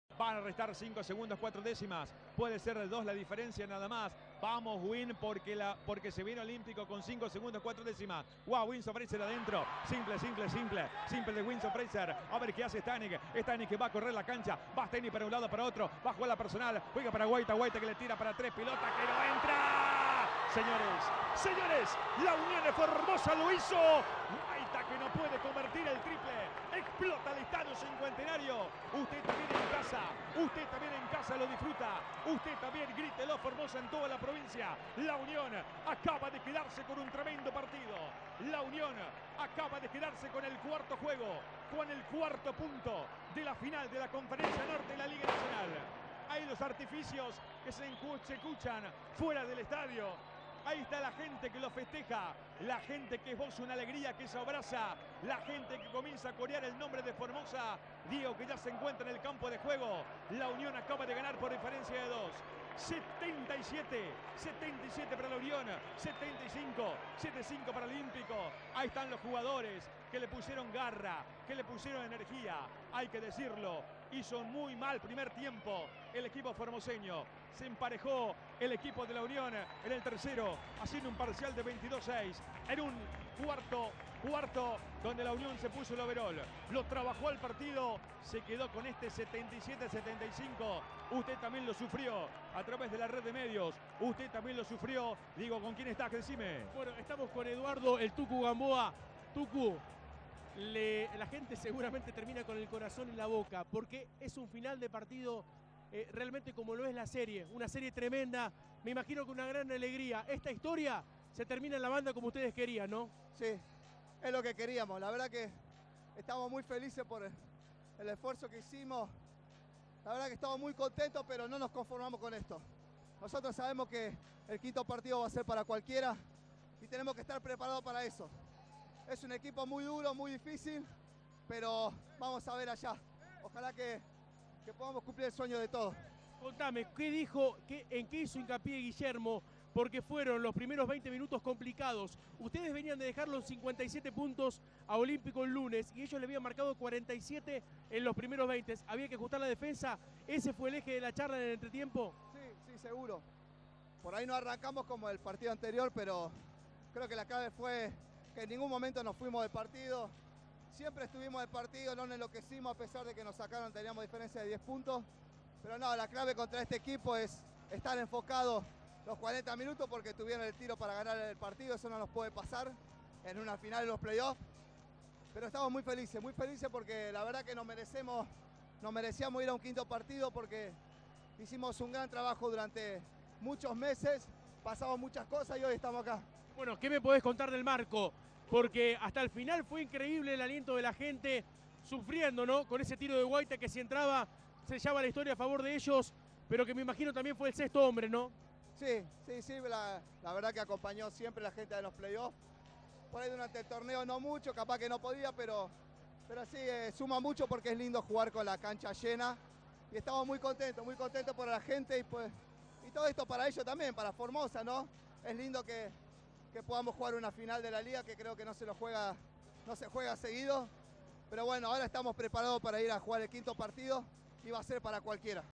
EN INFARTANTE FINAL, LA UNIÓN DERROTÓ A OLÍMPICO DE LA BANDA 77-75
La Unión le volvió a ganar en Formosa a Ciclista Olímpico de La Banda, esta vez por 77 a 75, y así igualó 2-2 la serie final de la Conferencia Norte de la Liga Nacional de Basquet (LNB), cuyo quinto partido se jugará el próximo sábado, a las 22, en Santiago del Estero.
El final fue muy emocionante ya que Diego Guaita, de la visita, tuvo el último tiro pegando la pelota en el borde del aro y saliendo para el festejo de la gran cantidad de formoseños que alentó en el Majestuoso Cincuentenario.